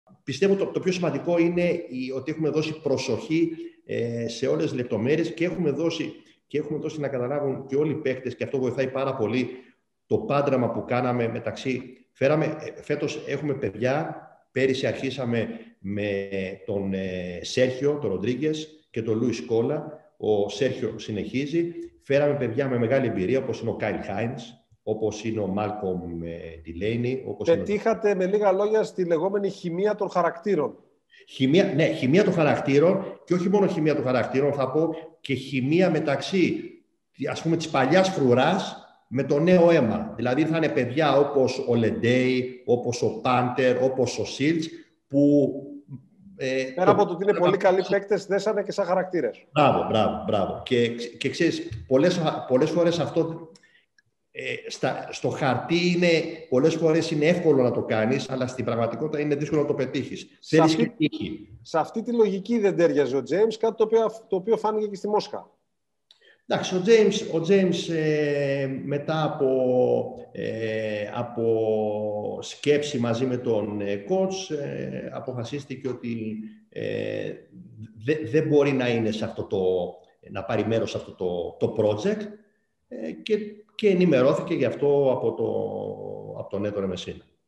Σταυρόπουλος στο gazzetta.gr: «Ο Μάικ Τζέιμς δεν μπορούσε να πάρει μέρος στο project της Αρμάνι»
Ο Έλληνας general manager της Αρμάνι Μιλάνου και εκ των στυλοβατών της ιστορικής πρόκρισης στο Final 4 της Euroleague, Χρήστος Σταυρόπουλος, μιλάει στο gazzetta.gr για την συνεργασία του με τον Μεσίνα, τις συζητήσεις του με τον Μάνο Παπαδόπουλο και τον ημιτελικό με την Μπαρτσελόνα και σχολιάζει την πορεία του ελληνικού μπάσκετ ως εξωτερικός παρατηρητής.